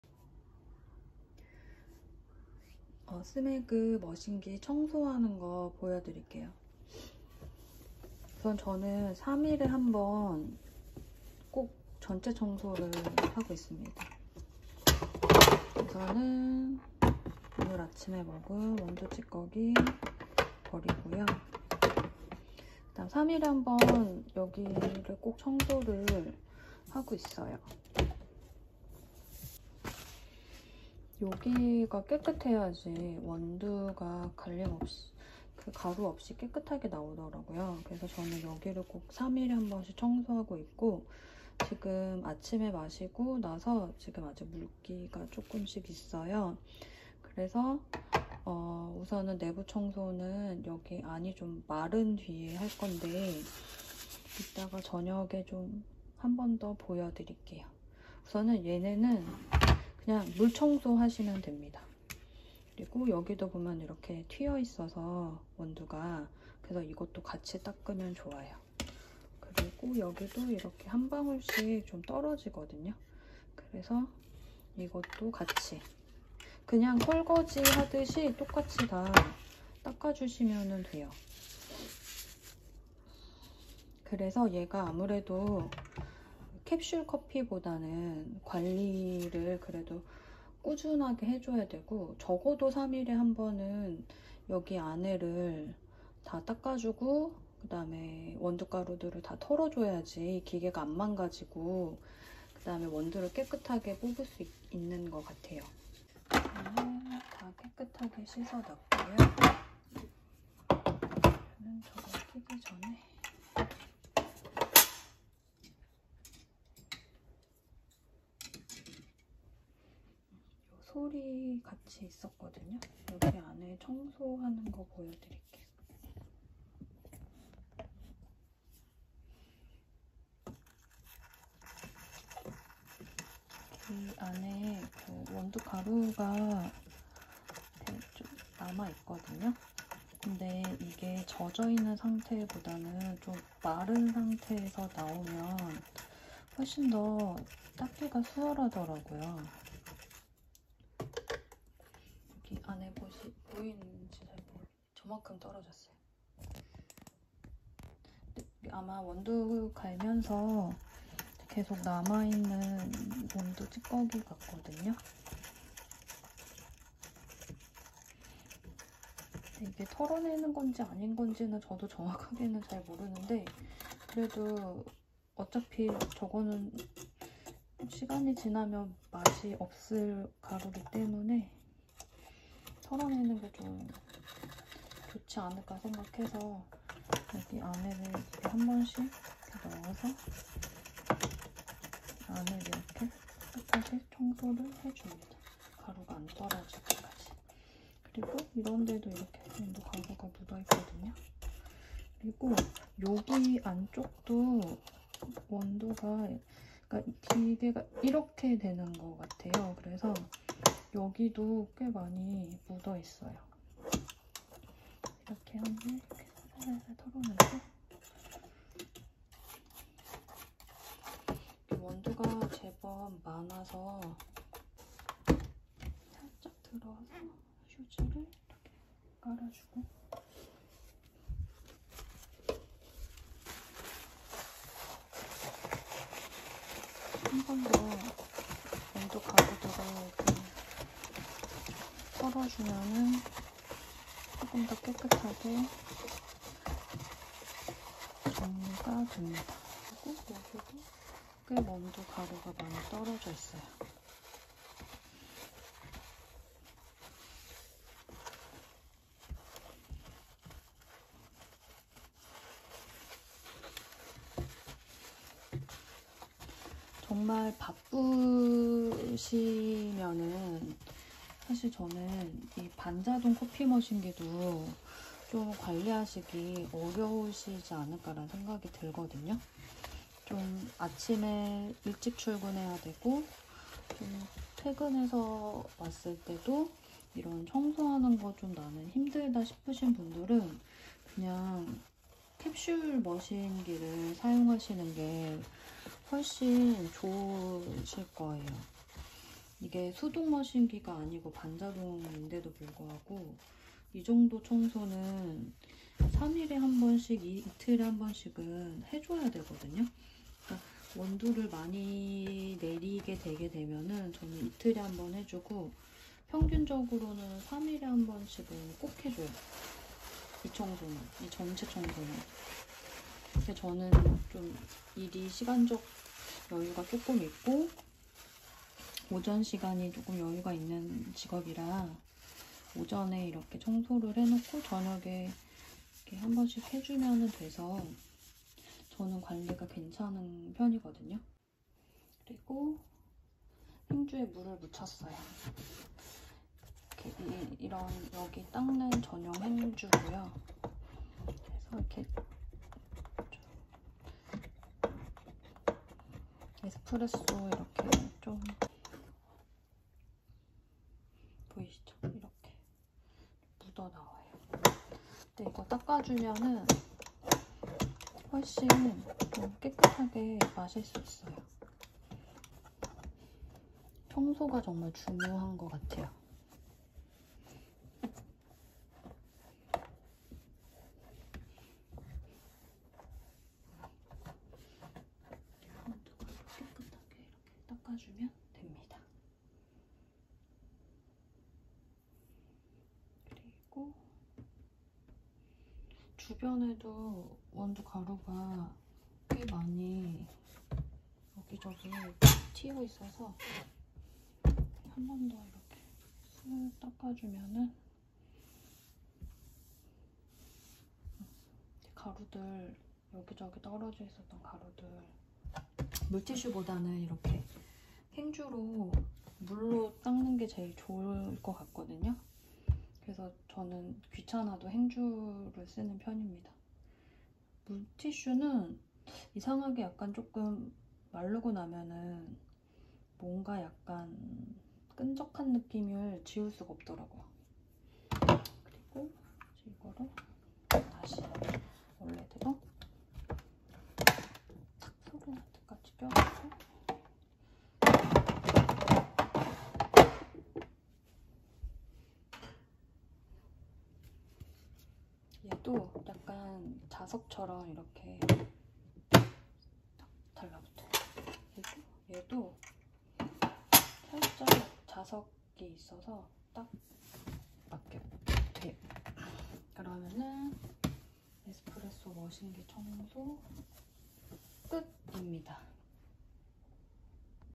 (일상) 스메그전자동커피머신기 청소하기! 빨리감기안해서 조금 길어요;;
스메그 동영상 조회수가 조금 높아서
청소영상도 찍어봤습니다~^^